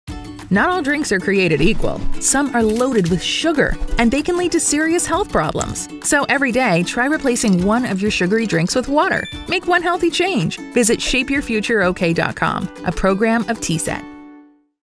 Limit Sugary Beverages | Healthy Living 101 | Shape Your Future | Oklahoma TSET
Discover the power of replacing sugary beverages with healthy, refreshing water! To learn more about the power of one healthy habit,